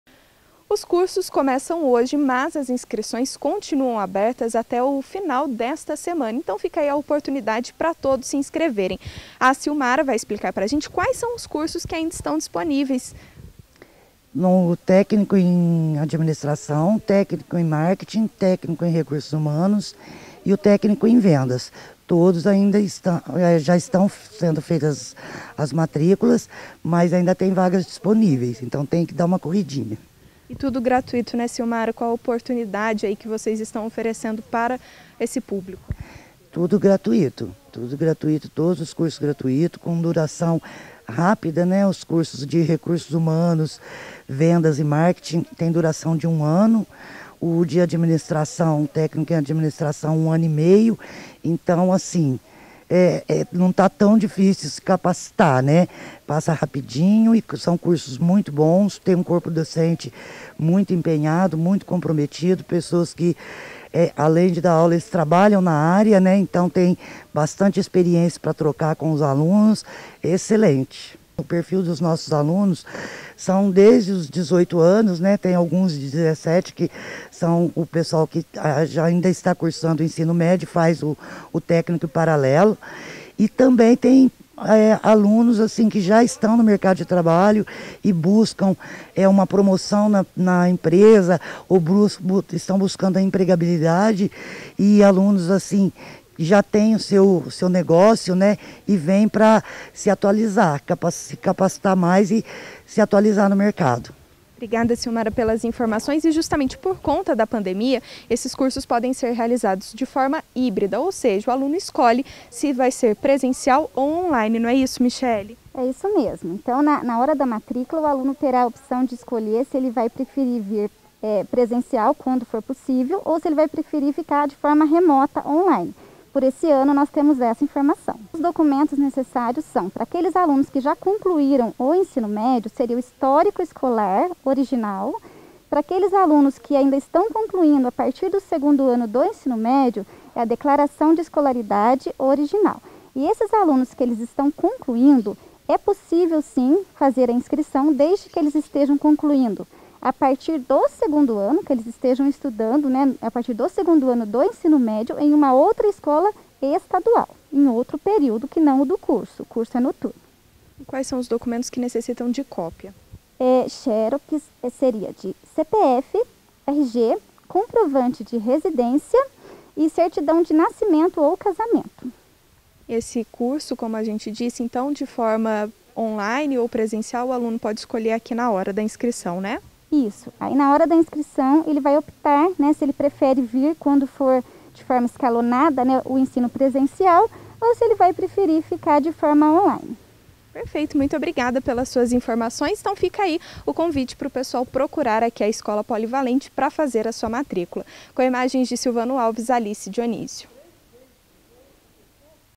CURSO TÉCNICO POLIVALENTE
A Escola Estadual Professor Arlindo Pereira (Polivalente) está com inscrições abertas para os cursos técnicos gratuitos de administração, recursos humanos, marketing e vendas.